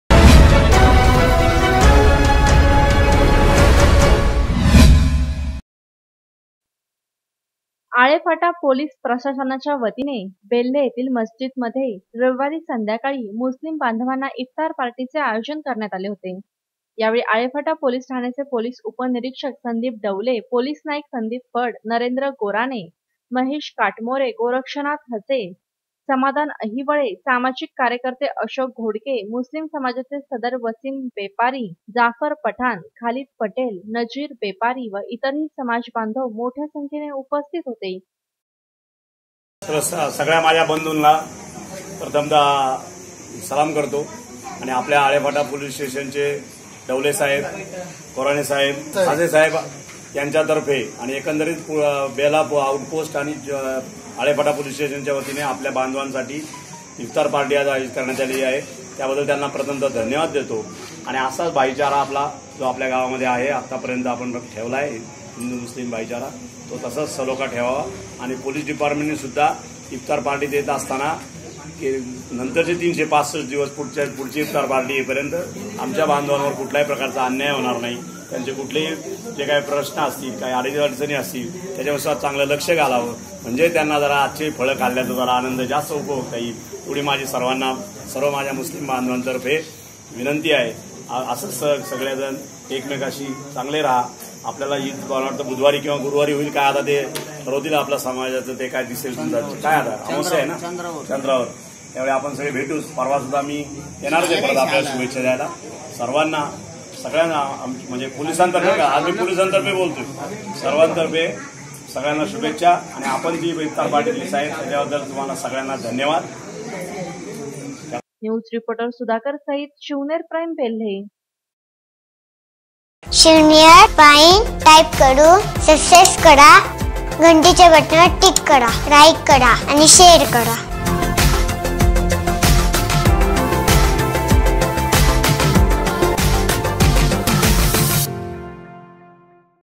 आळेफाटा पोलीस प्रशासनाच्या वतीने मुस्लीम बांधवांना इफ्तार पार्टीचे आयोजन
आळेफाटा पोलीस_प्रशासनाच्या_वतीने मुस्लीम बांधवांना